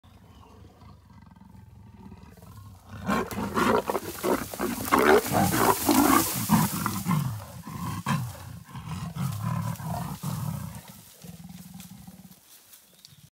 Two Leopards Clash in Epic Showdown!
In this video from our archives, two powerful leopards fight in a thrilling encounter.